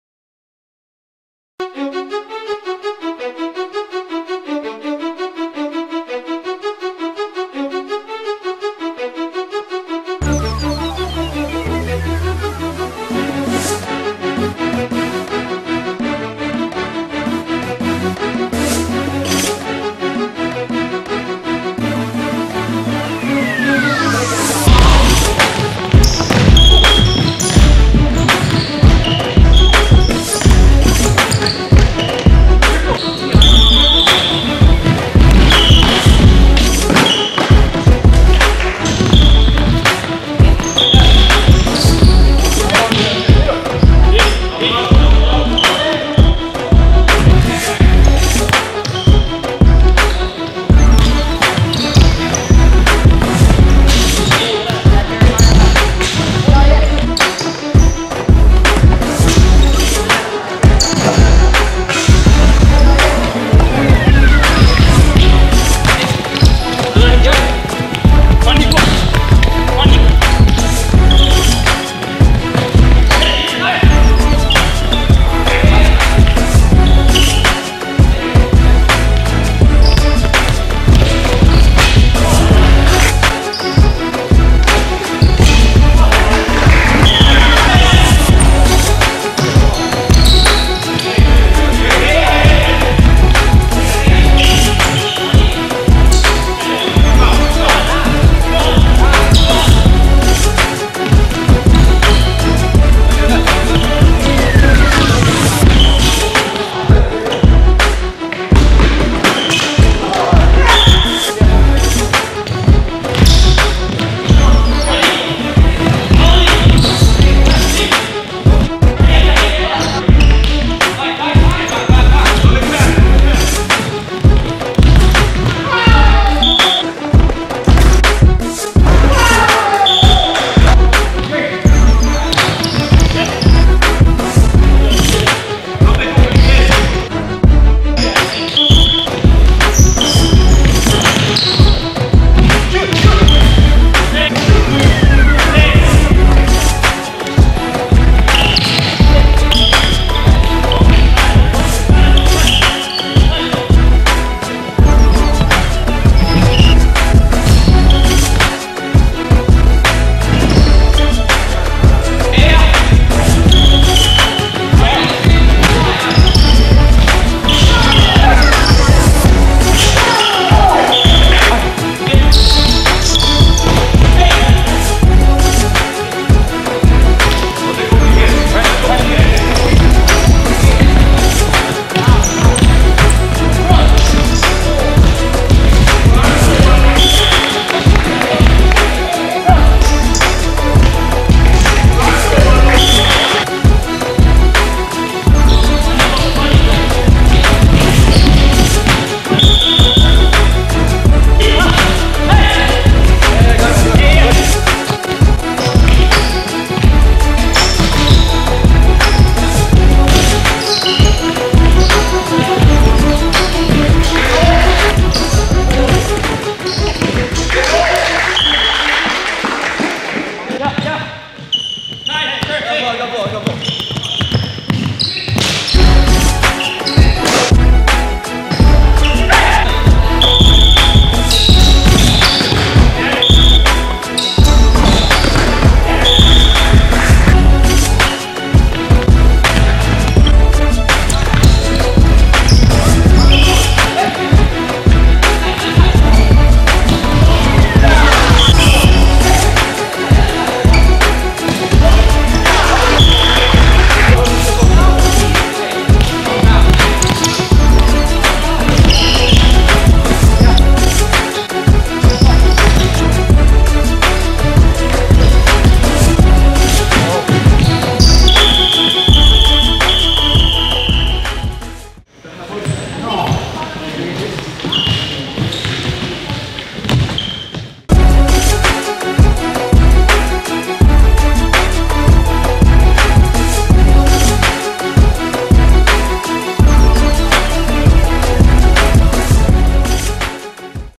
STUNNING SOLO GOALS, BICYCLE KICK, STIFF SAVES, SKILLS AND ACTION!!!
PREMIER LEAGUE FUTSAL
@ CORE Lifestyle and Recreation Complex
Kitchener, Ontario, Canada